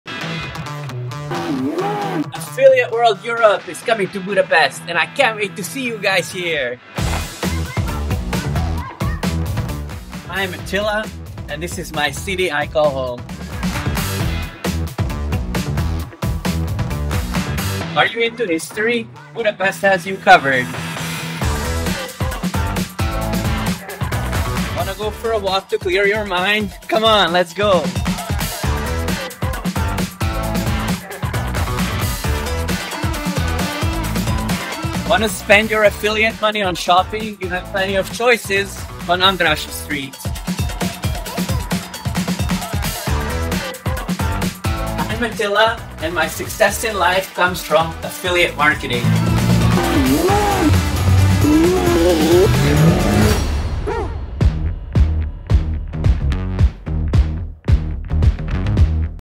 Join the #1 Affiliate Marketing Event | Save €900+ Today
Affiliate World Europe is back this 5-6 September 2024.

But this time in a whole new city, Budapest 🇭🇺

✅ Learn from the top 1% of affiliate marketers and entrepreneurs.
35+ speakers revealing game-changing strategies on:
🔥 Meta Ads
🔥 TikTok Ads
🔥 Google Ads
🔥 Native Ads
🔥 Affiliate Marketing’s Hottest Topics

✅ Networking with 7,500+ attendees and seal deals with 600+ advertisers, traffic sources, and networks:
👋 Advertisers clocking $1M+ monthly
👋  Marketers generating over $100k every month
👋  Pioneers of lead generation
👋  Rapidly growing Shopify stores
👋  And an array of industry trailblazers

✅ Have fun at happy hours and epic afterparties!

If you’re looking to foster strong high-value relationships, you’ll fit like a glove at Affiliate World.

⏰ But hurry, prices are increasing 3x weekly